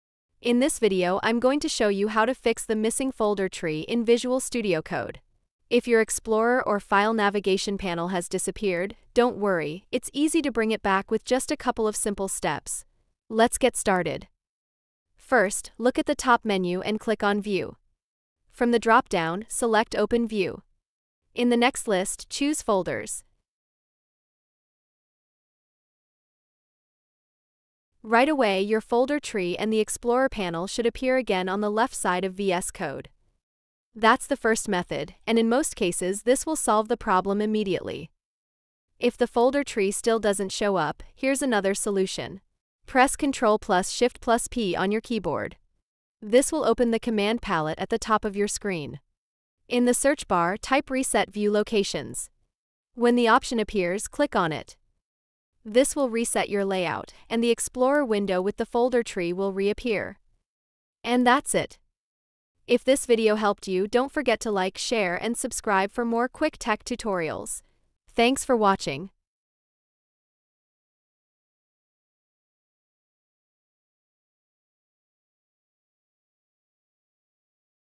How to Fix Missing Folder Tree in VS Code | File Explorer Not Showing
Are you facing the issue of a missing folder tree or file explorer in Visual Studio Code? In this video, I’ll show you two easy methods to fix the missing folder tree in VS Code.